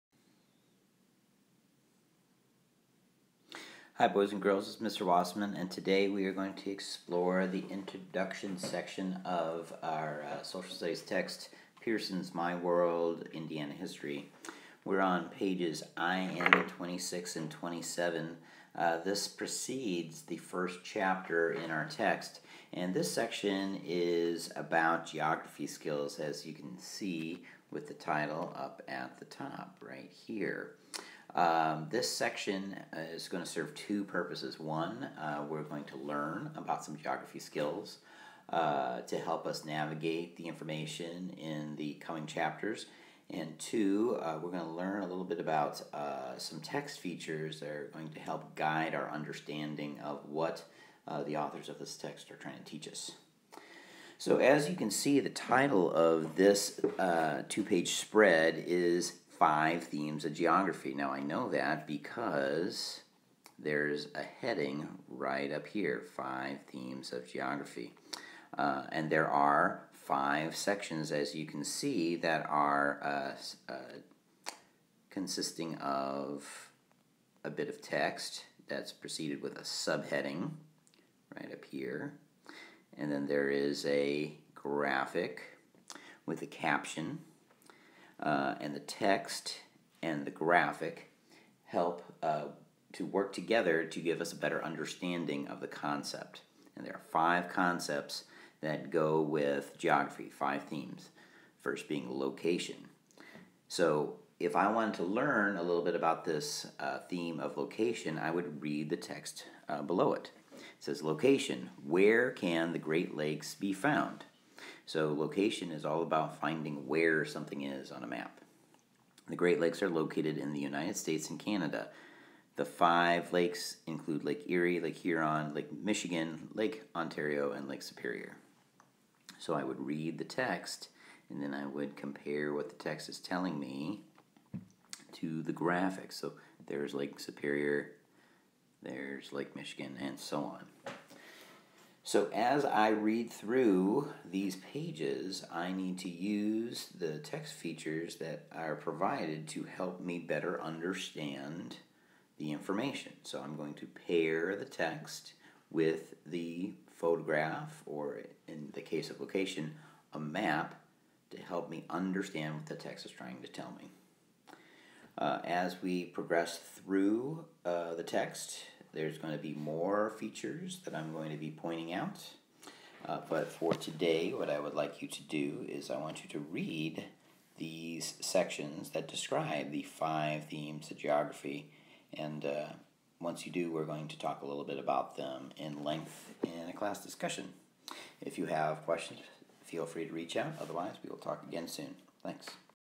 Pearson My World Social Studies, 4th Grade, IN 26-IN 27: "Five Themes of Geography"
Mr. Wasemann describes how to navigate the 4th grade social studies text by exploring the 5 themes of geography while utilizing text features such as headings, graphics and captions.